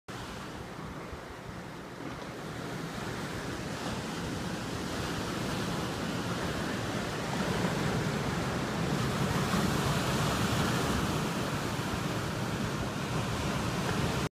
pelican fishing in the surf at sunset, Oceanside CA
pelican fishing in the surf at sunset